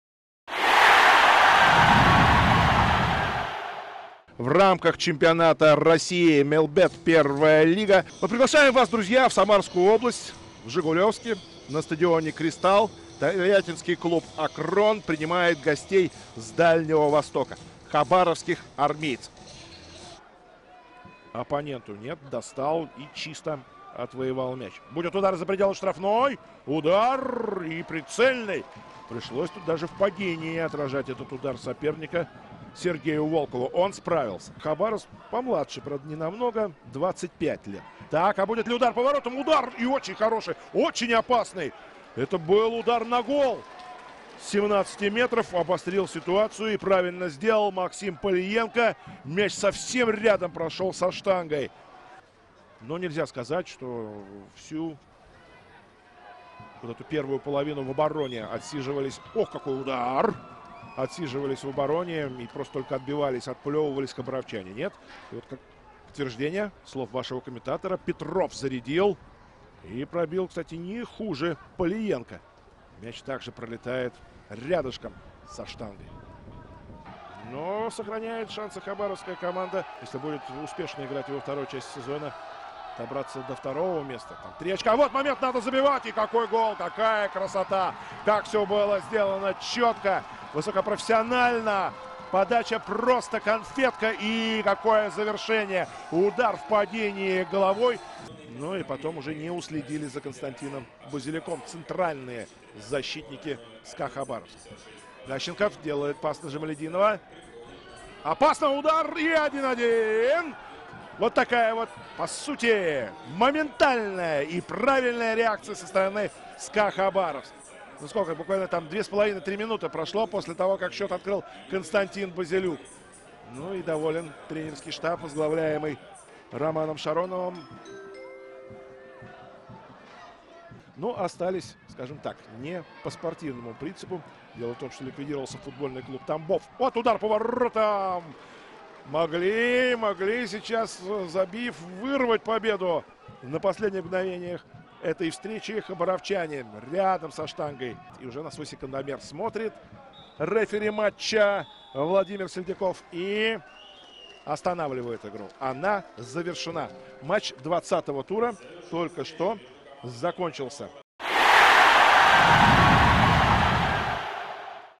Обзор матча «Акрон» — «СКА-Хабаровск» | 20 тур МЕЛБЕТ-Первой лиги
Лучшие моменты из игры «Акрона» (Тольятти) и «СКА-Хабаровска» (Хабаровск) 27 ноября 2022.